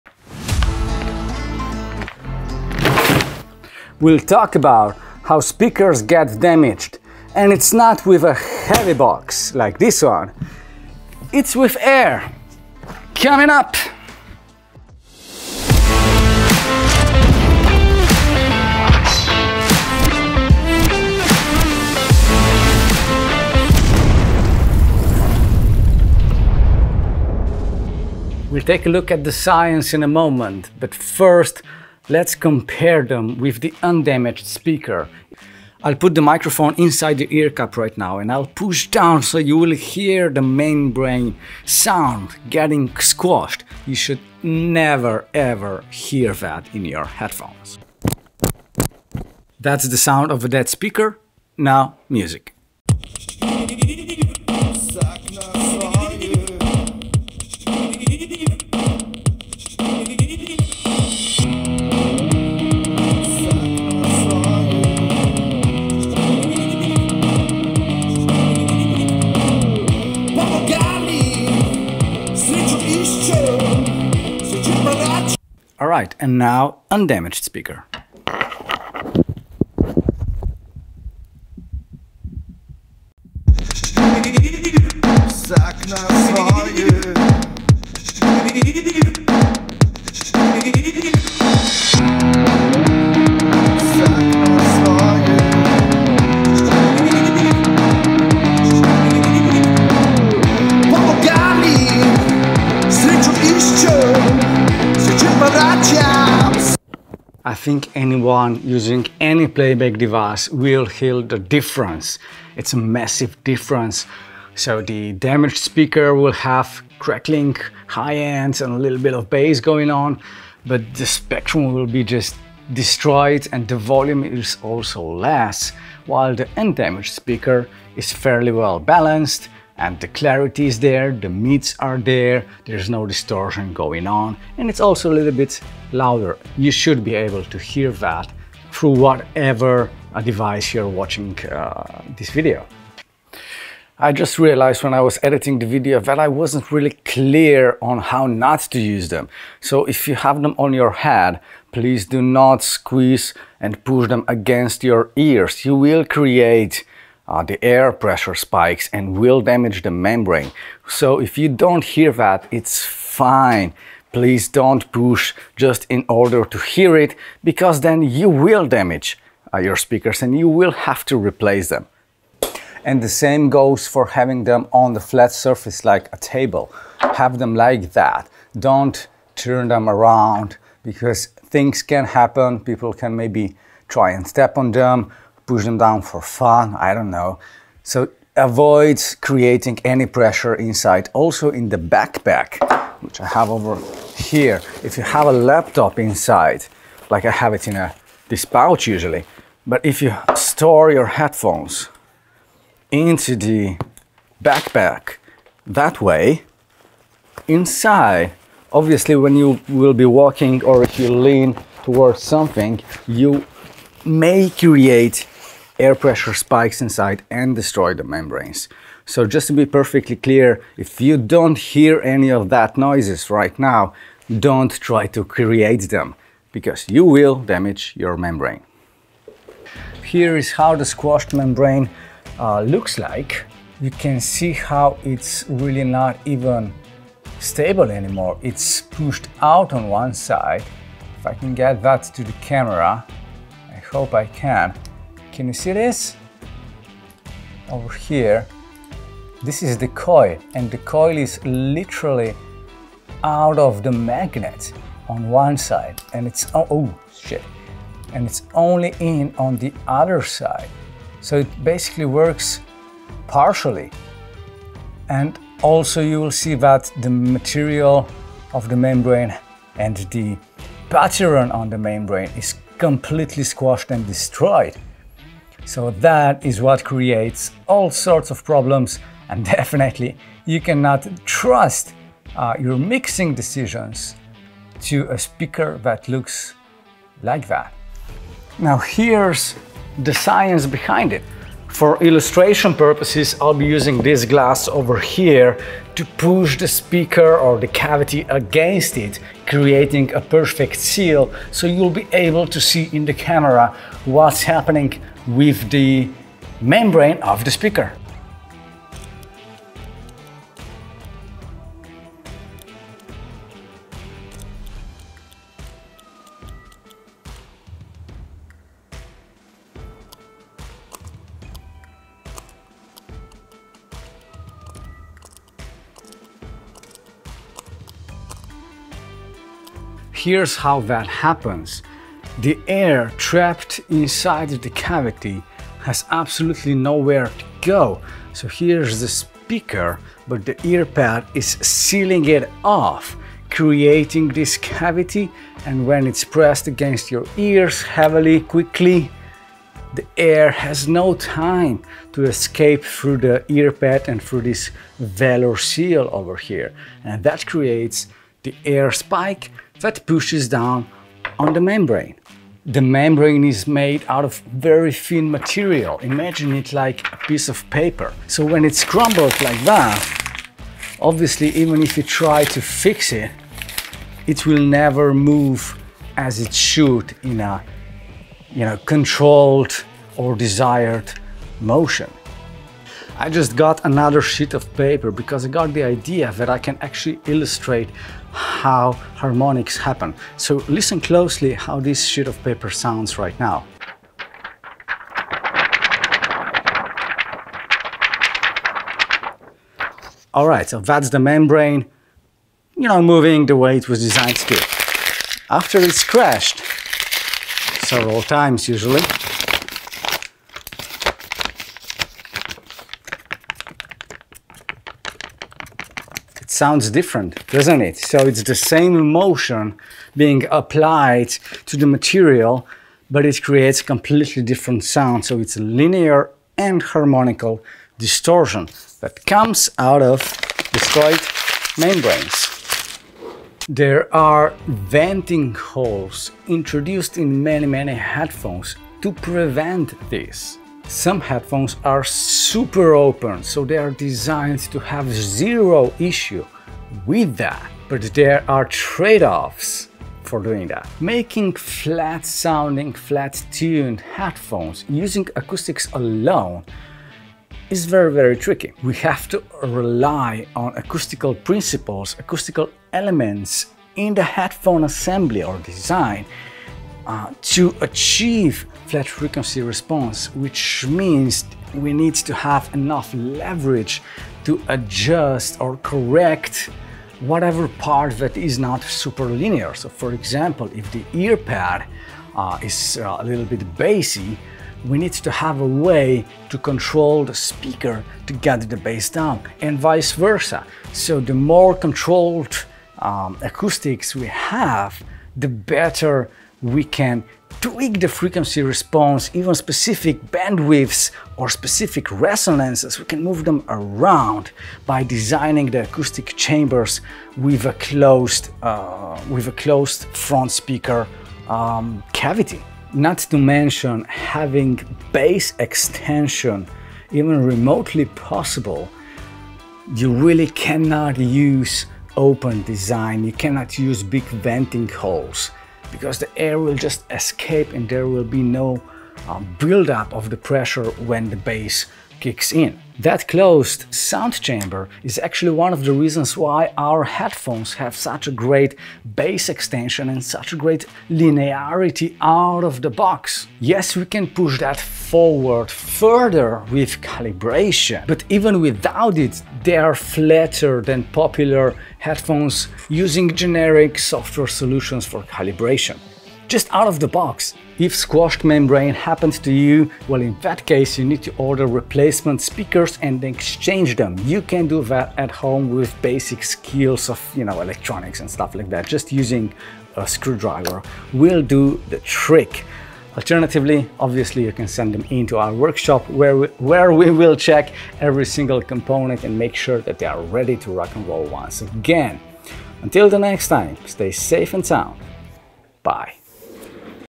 How you can destroy your perfectly flat sounding pair of studio headphones without knowing it.
In this EP No. 38, we'll explore how air, not impact, destroys speakers. After comparing their sound with undamaged ones, it's clear they're compromised. A short sound demo is included, so you can take alisten yourself.

Opening them reveals a crumpled membrane, damaged by air pressure spikes when the cavity shrinks too quickly. That is always a user's error in handling. It's what happens to most any headphones. So be careful regardless of the model you have. Vent holes prevent this, but have trade-offs like impacting bass extension. Our headphones' closed chamber yields a flat frequency response, further enhanced by calibration. For damaged membranes, users can replace speakers themselves or send them for professional repair.

00:00 Intro
00:52 demo with squashed membranes
01:20 demo with undamaged speakers
02:20 important warning
04:07 close ups of a damaged speaker
05:09 closeups demonstration of the speaker movement under airpessure spikes
05:30 scientific explanation
06:30 illustration of sound changes after squashed membrane
07:10 Venting holes trade offs and outro